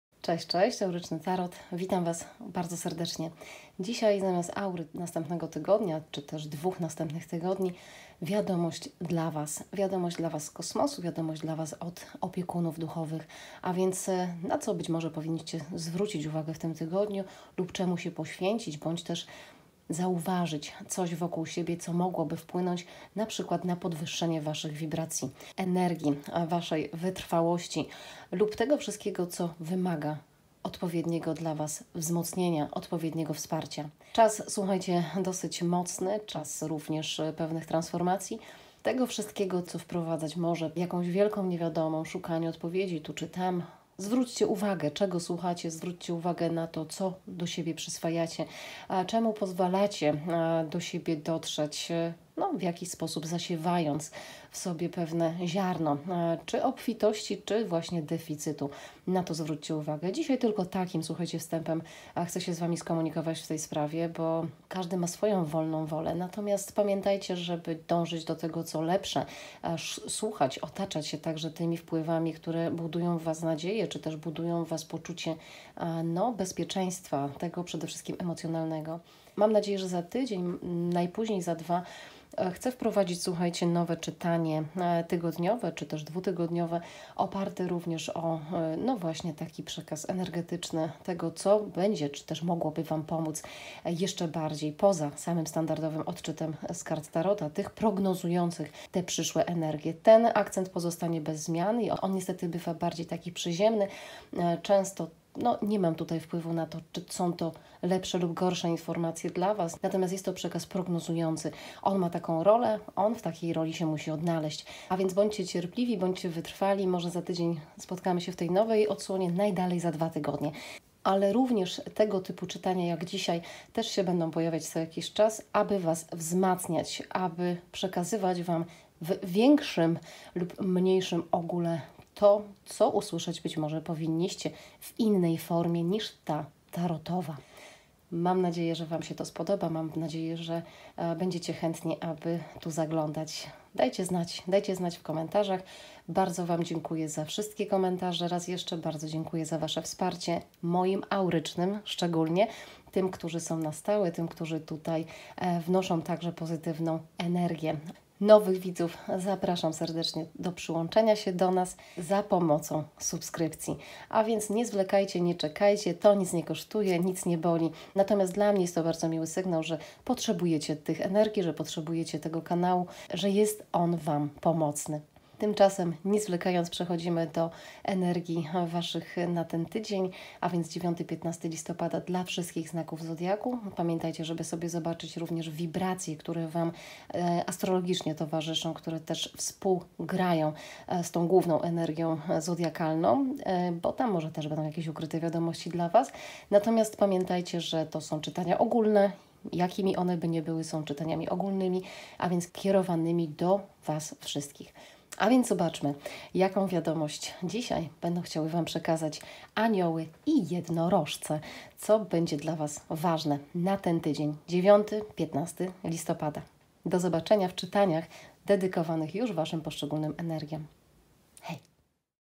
INTRO - WZMOCNIENIE AURY na 9-15 listopada dla wszystkich znaków zodiaku
WIADOMOŚĆ WZMOCNIENIA AURY na 9-15 listopada
POCZĄTEK CZYTANIA: 03:40

Horoskop auryczny, czytanie ogólne, wiadomość.
Karty:  Diana Cooper Spojrzenie Anielskie
Diana Cooper Karty Jednorożców